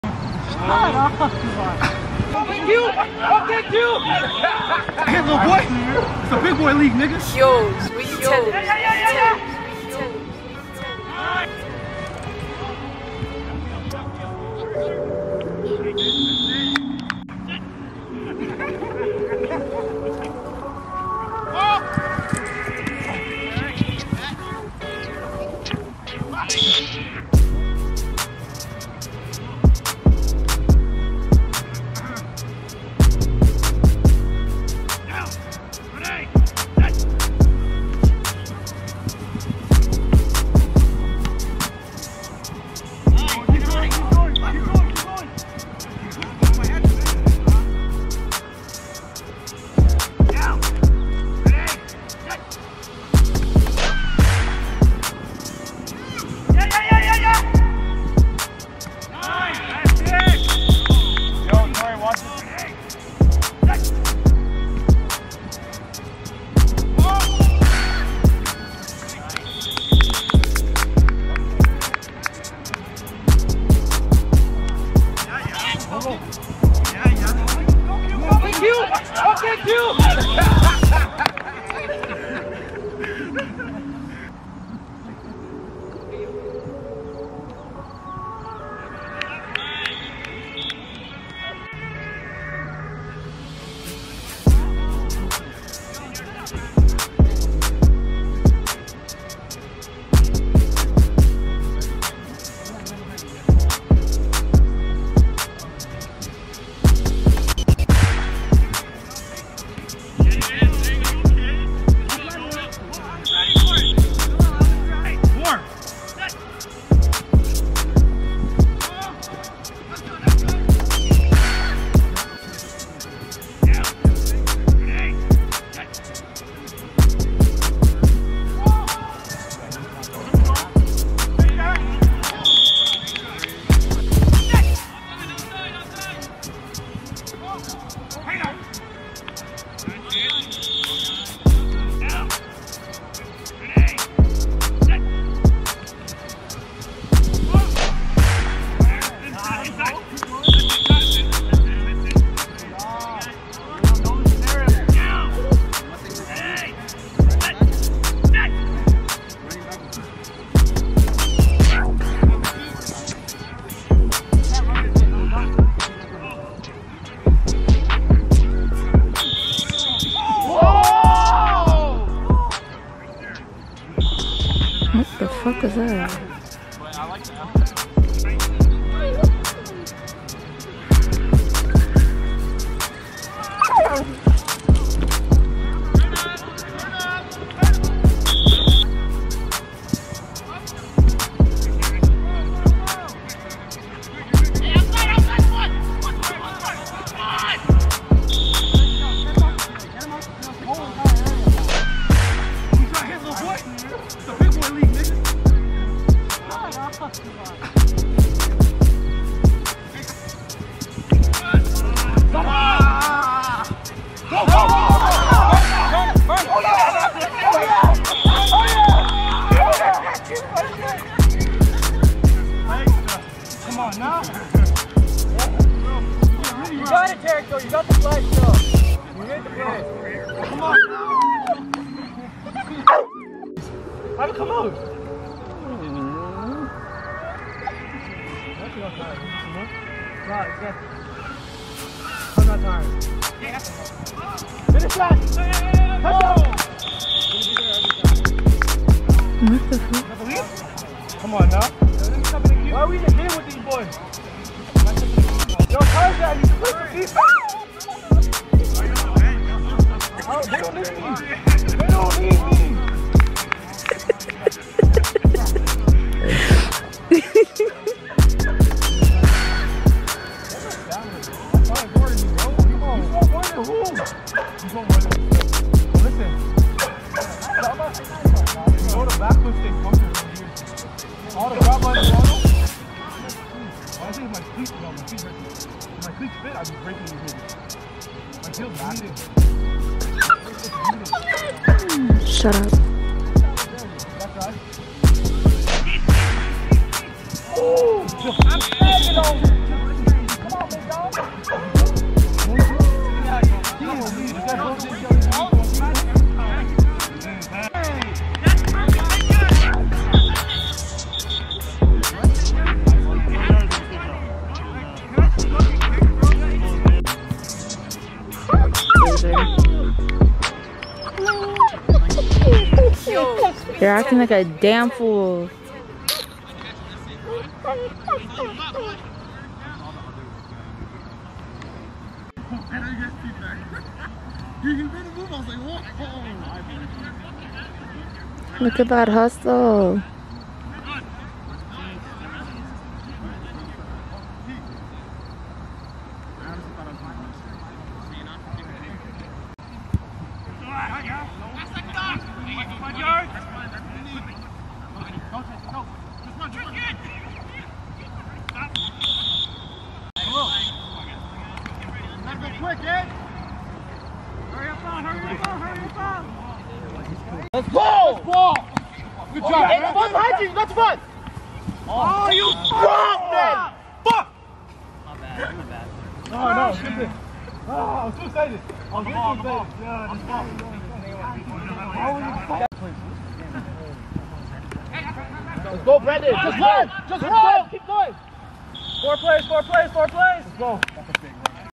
FLAG FOOTBALL going crazy
lemme know if i should post more flag football videos, they really be talking the most shit

COMMENT .. all comments are welcomed

eastside lost by 1 touchdown